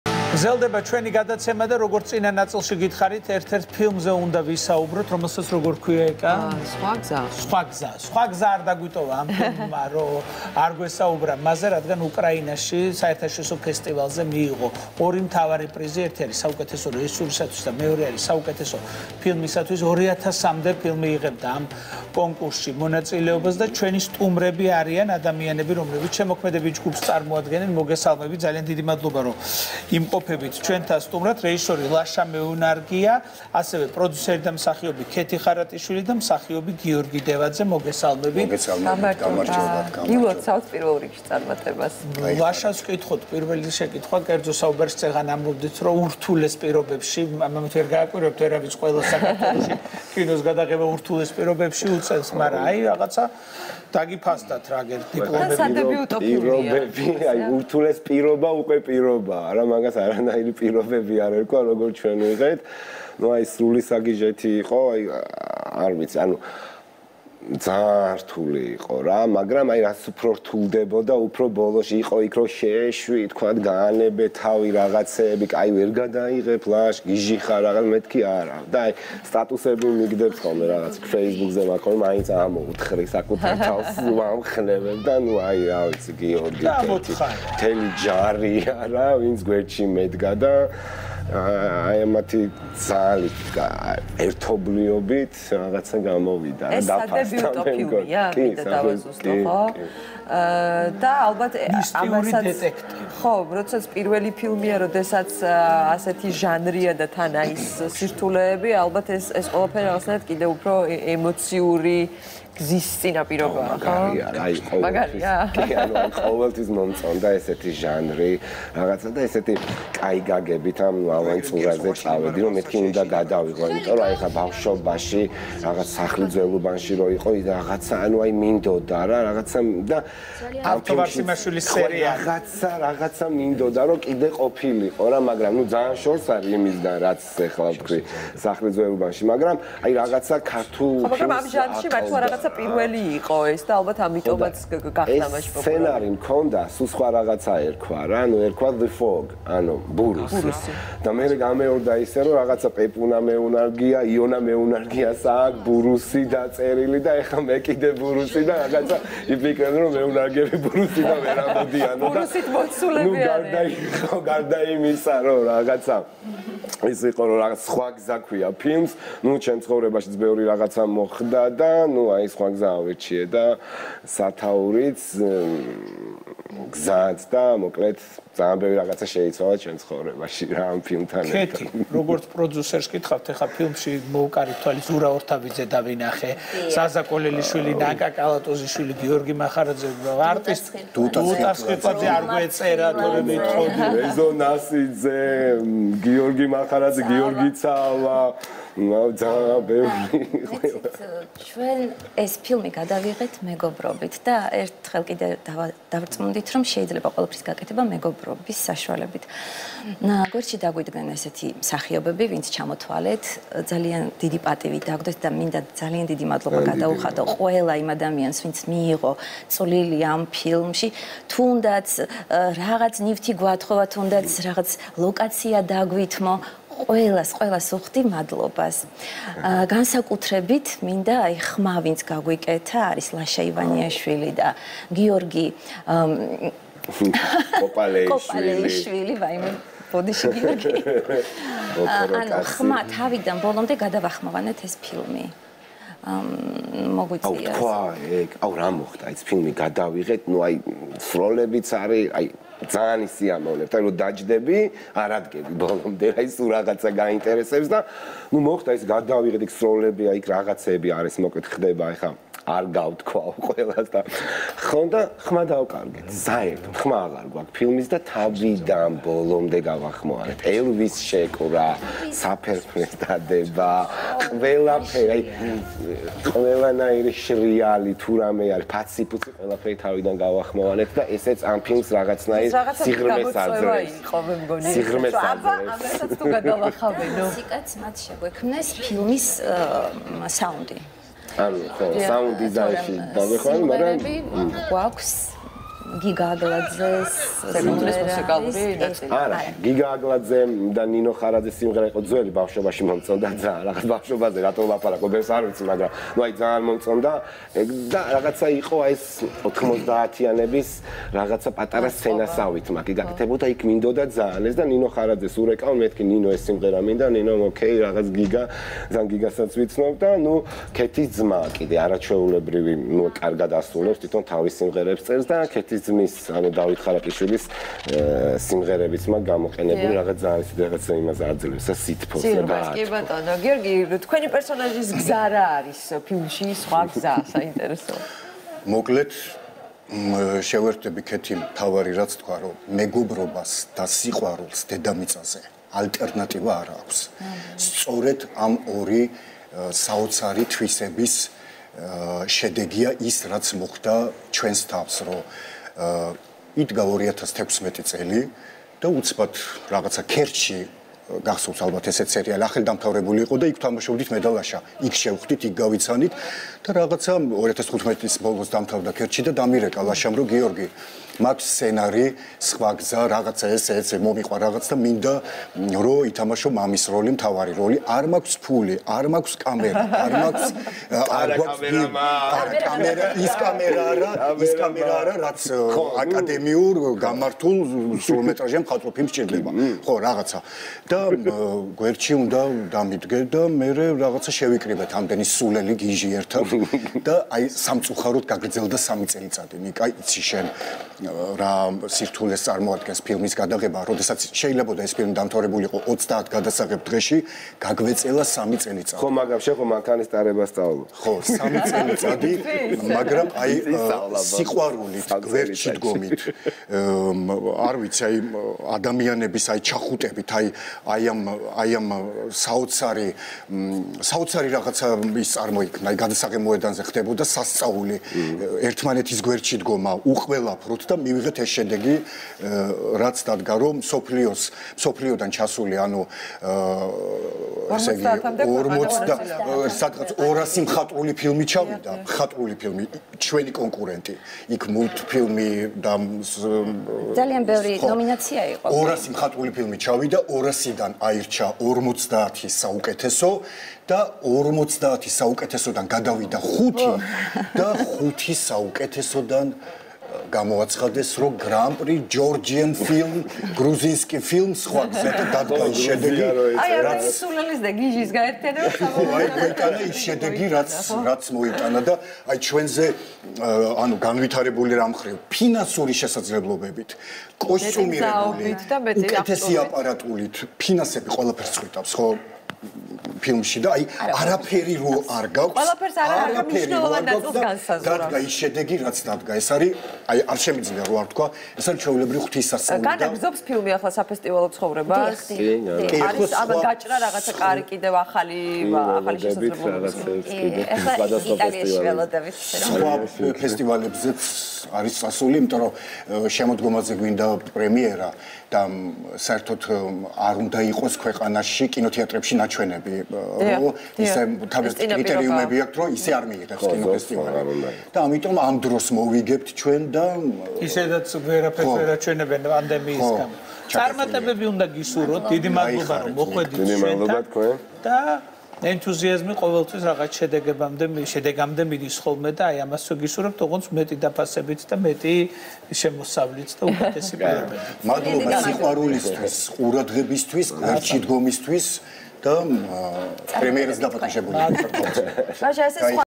ლაშა მეუნარგიას ფილმი „სხვა გზა“ კიევის საერთაშორისო ფესტივალზე
ფილმი 5 საუკეთესოთა შორის მოხდა და გაიმარჯვა ორ ნომინაციაში - საუკეთესო რეჟისორი და საუკეთესო ფილმი